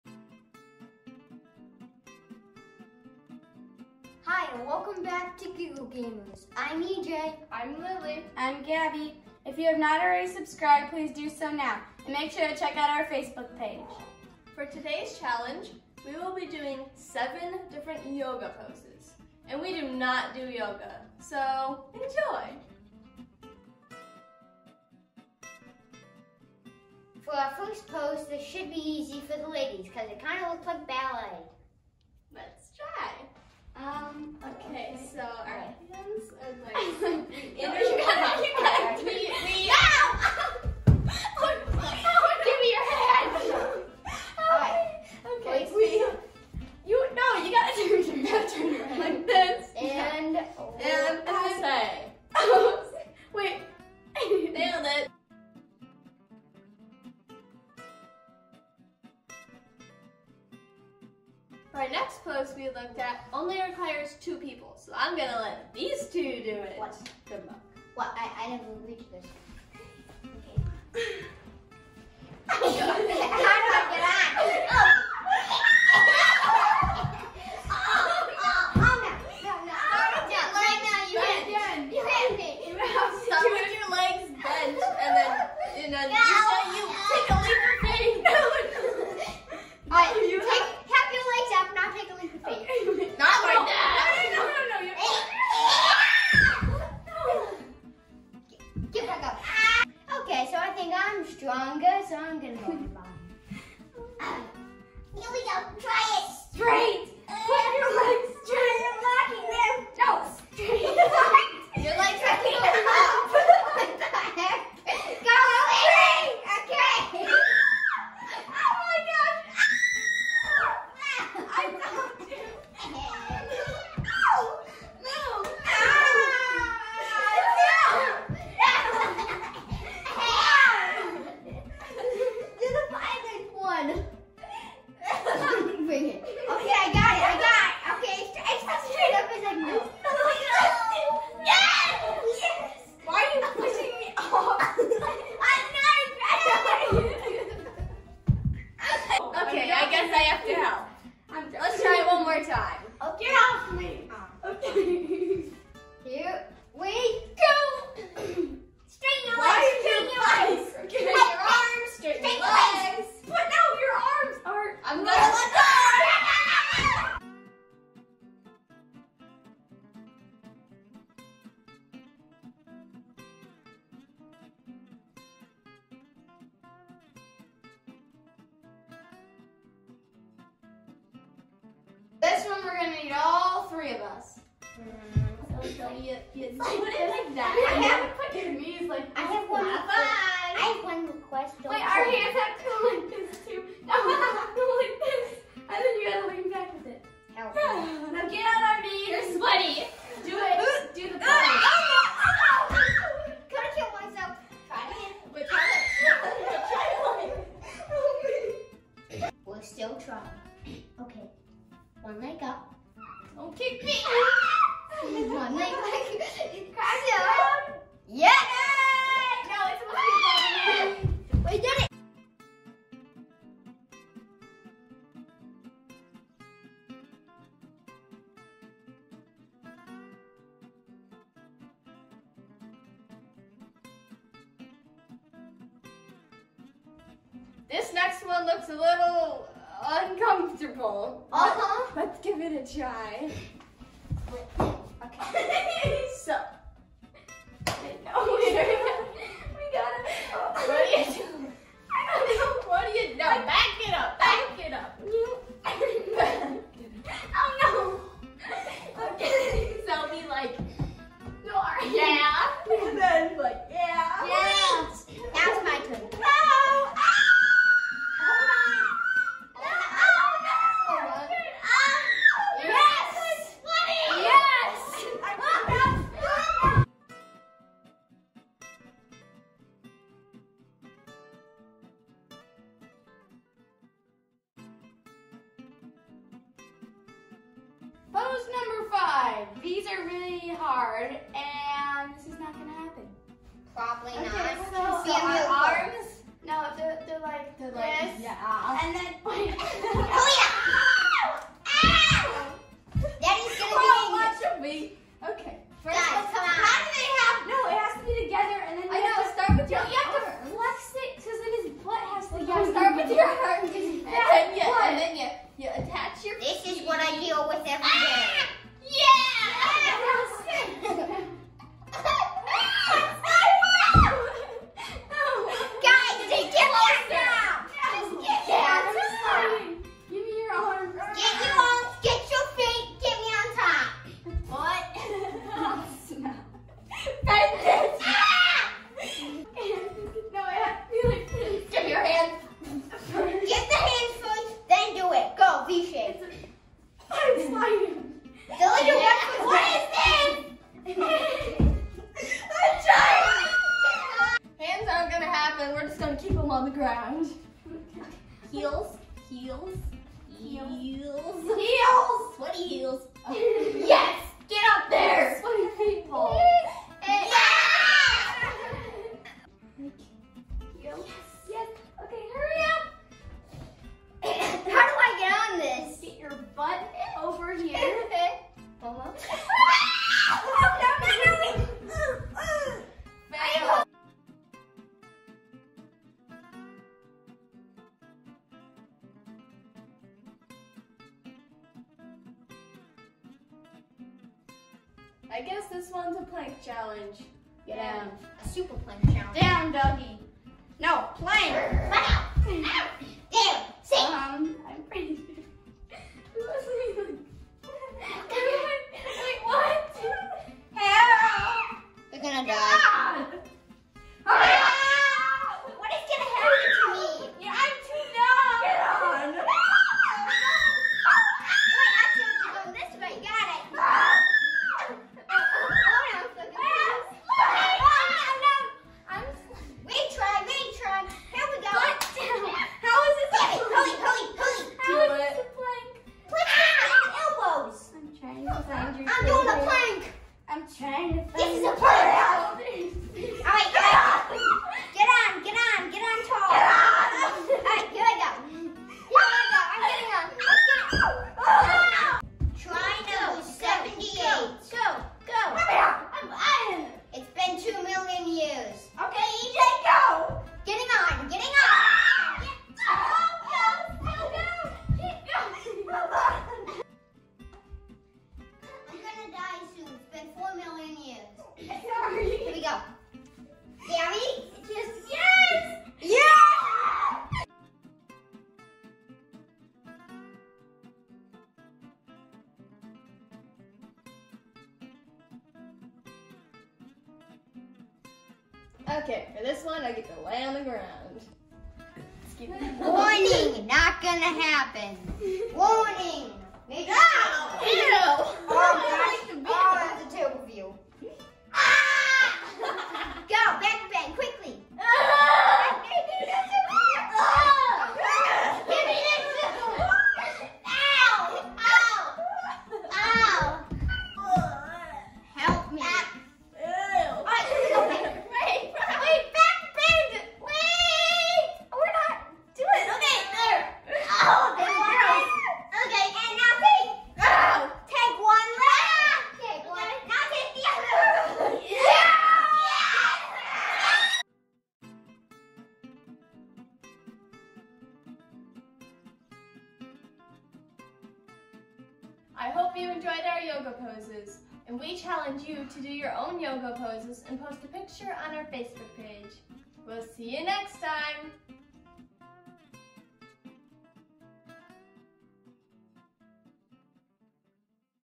Giggle Gamerz Yoga Challenge
Watch as the Giggle Gamerz attempt to do some group yoga poses.